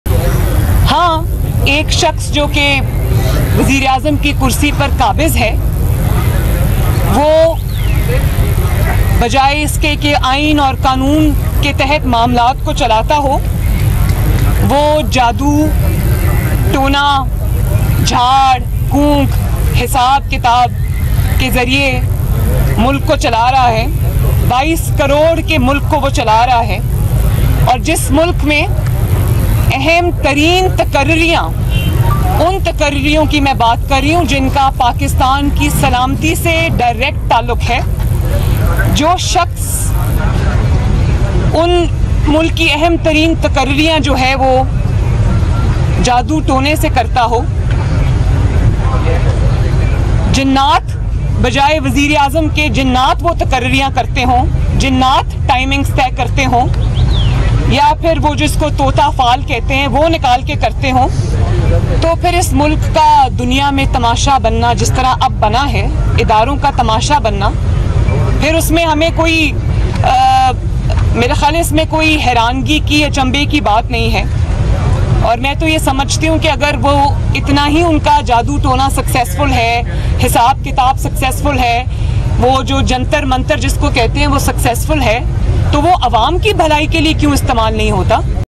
Imran Khan Is Running The Country Through Sorcery: Maryam Nawaz
PML-N Vice President Maryam Nawaz has said that Imran Khan is running the country through sorcery, corruption scandals, and incompetence.